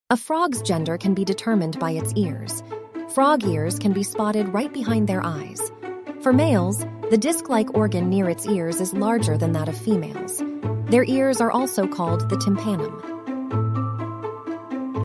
How to Identify Frog's Gender
There is an ear covering called a tympanum. On females, this disc is the same or smaller circumference as her eye. The tympanum of ear covering on a male is generally bigger than its eyes.